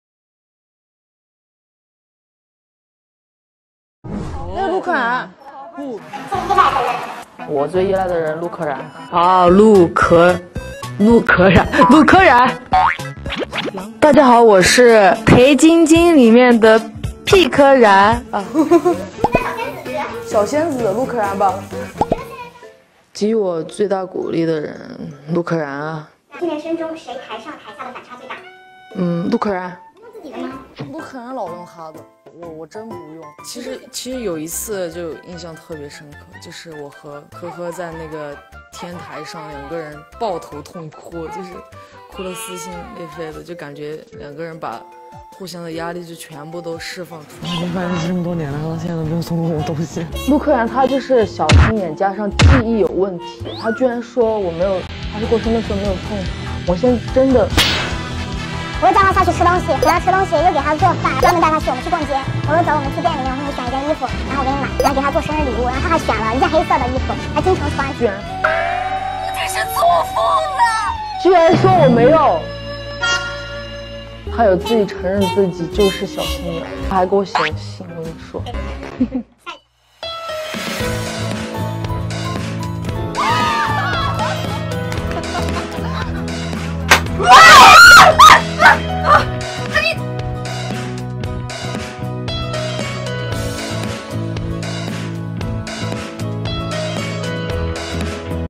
Lin Fan mentions Lu Keran in her interviews (凡柯)
突然好想凡柯~~Suddenly miss FanKe's moments. So here is a compilation of the number of times Lin Fan mentions Lu Keran in her interviews.

-Click on CC for subtitle